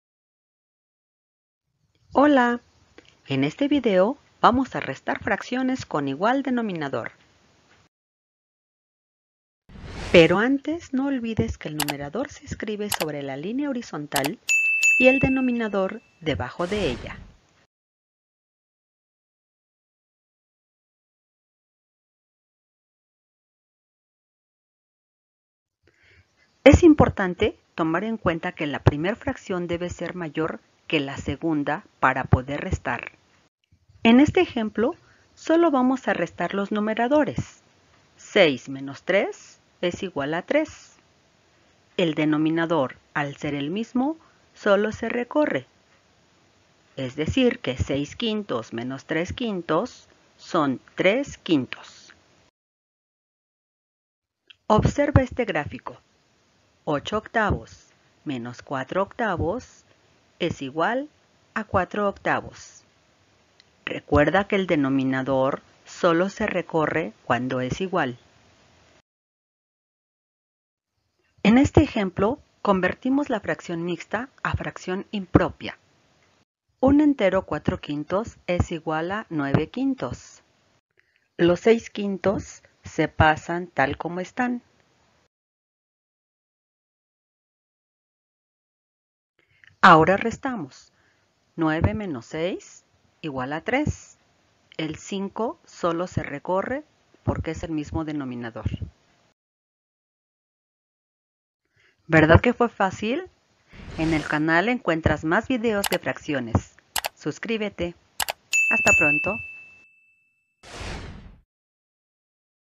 Resta de fracciones con igual denominador
En este video te explico resta de fracciones con igual denominador, numerador, denominador, fracción mixta a fraccion impropia y como se recorre el denominador.